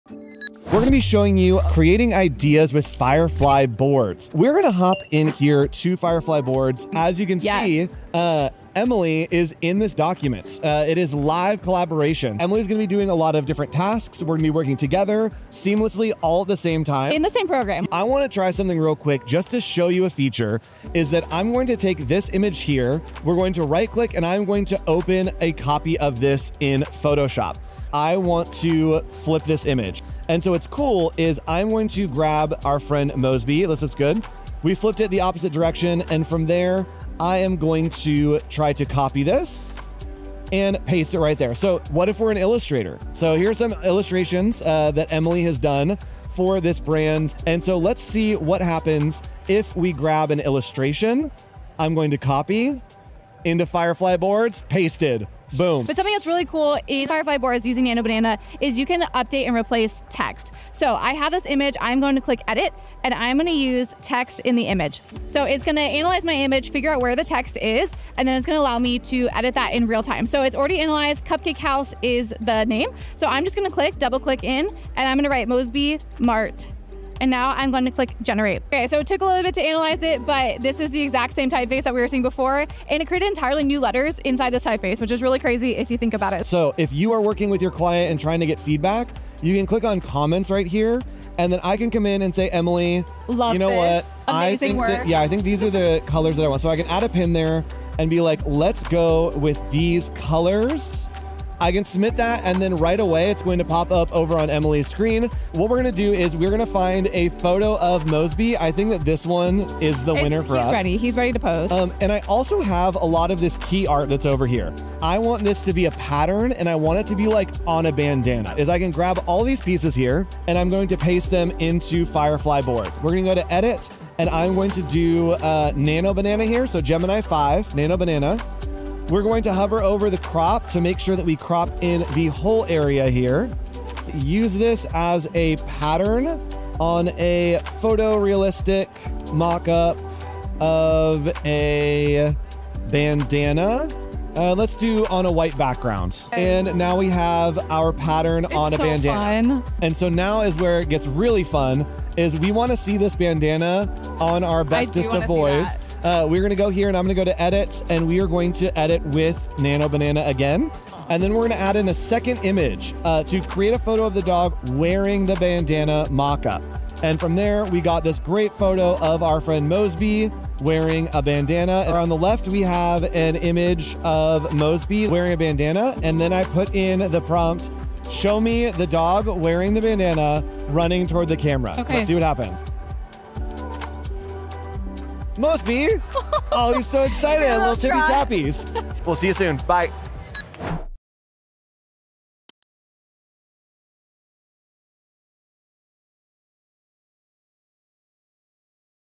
Co-Creating in Firefly Boards!
Join Emily and Andrew while they explore how Firefly Boards brings ideas to life through real-time collaboration. See how images are flipped in Photoshop, Illustrator artwork is pulled in for quick experimentation, text is updated with Nano Banana, feedback is shared instantly, and a custom pattern becomes a bandana for the ever-stylish Mosby! 🐶